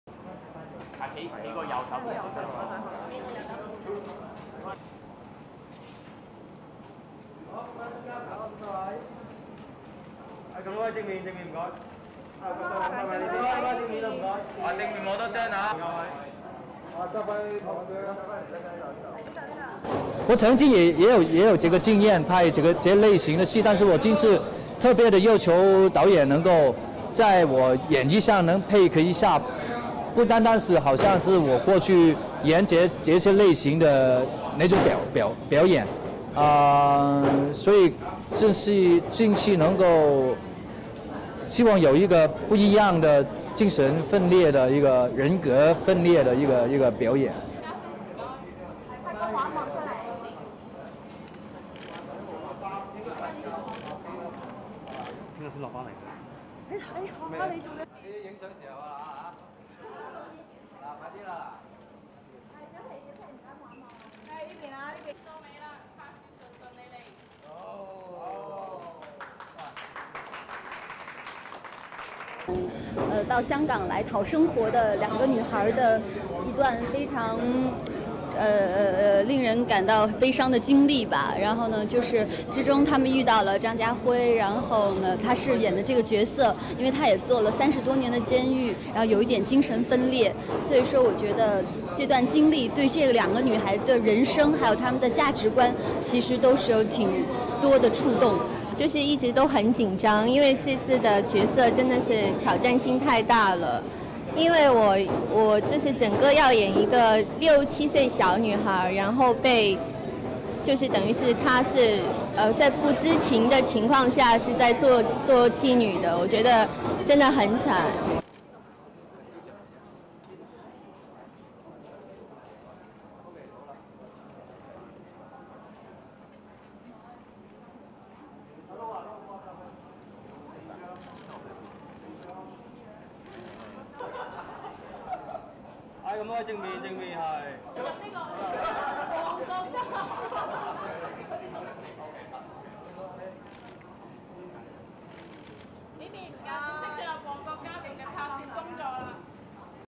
'To Live and Die in Mongkok' starts filming in Hong Kong
AP Entertainment
Hong Kong, 30 July 2009
1. Wide shot cast gathering in front of altar table
2. Wide shot of press
3. Wide shot all cast and staff standing for photo call
4. Medium shot Meng Yao, Nick Cheung Ka-fai, Chen Li-yun
5. Close shot photographers
6. SOUNDBITE (Mandarin) Nick Cheung Ka-fai, Actor:
"I used to play the character like that. But I asked for the director to coordinate the character and hoped that I would perform in a different method. I hope that I can translate a person who has suffered from mental disorder and split personality into a new realm."
7. Wide shot Nick Cheung Ka-fai, Chen Li-yun putting incense into burner
8. Medium shot Photographers
9. Medium shot pull out Meng Yao, Mo Xiao-qi putting incense into burner
10. Medium shot pull out to cast cutting suckling pig and clapping hands
11. SOUNDBITE (Mandarin) Mo Xiao-qi, Actress:
"It's about the sad experience of two girls coming to Hong Kong. They met the character played by Nick Cheung Ka-fai who was imprisoned for thirty years and suffered from a split personality. There are lots of emotions derived from two girls' life and values."
12. SOUNDBITE (Mandarin) Meng Yao, Actress:
"I was very nervous since this character really gives me challenge. I have to play a retarded girl who has the abilities of a 6 or 7 year old child. It's a sad story."
13. Medium shot pull out Nick Cheung Ka-fai, Meng Yao, Mo Xioa-qi burning mock money
14. Wide shot of press
15. Medium shot  pull out All cast holding banner for photo call
STORYLINE
AWARD-WINNING CAST 'LIVE AND DIE IN MONGKOK'
Two Hong Kong Film Award winners are teaming up for Wong Jin's new movie 'To Live And Die in Mongkok'.
Acting stars Nick Cheung Ka-fai and Paw Hee-ching will both appear in the director's latest movie.
Filming started in Hong Kong's Causeway Bay on Thursday (30 JUL 2009) with a burning-incense ceremony that hopes to bring good luck and success to the production.
'To Live and Die in Mongkok' will show audiences the hardships suffered by the lower classes in the area and the difficulties they overcome to simply survive.
Nick Cheung Ka-fai, the latest Hong Kong Film Awards best actor winner, plays a man who is set free after thirty-years behind bars.
He also suffered from a mental disorder and struggles with social acceptance:
"I hope that I can translate a person who has suffered from a mental disorder and split personality into a new realm." said Cheung.
While best actress winner Pau Hee-ching plays his mother, who also has to take care of her retarded daughter from Mainland China.
Meng Yau, another star who was successful at the Hong Kong film awards, plays Paw's daughter and said the character is a real challenge for her.
As 'To Live and Die in Mongkok' features such a critically-acclaimed cast the feature will be hoping to garner plenty of nominations during next year's award season.